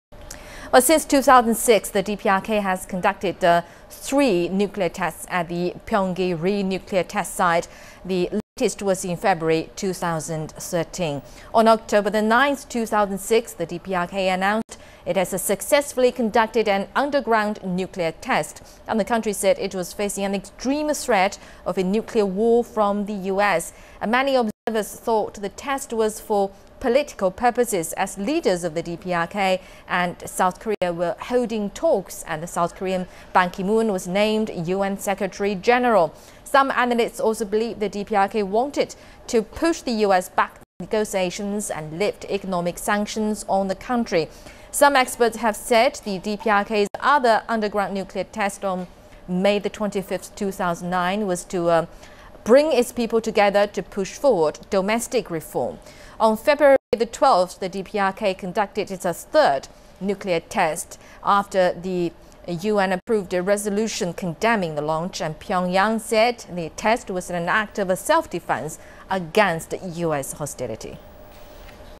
DPRK has conducted three nuclear tests since 2006
Since 2006, the DPRK has conducted three nuclear tests at the Punggye-ri nuclear test site, the latest was in February 2013.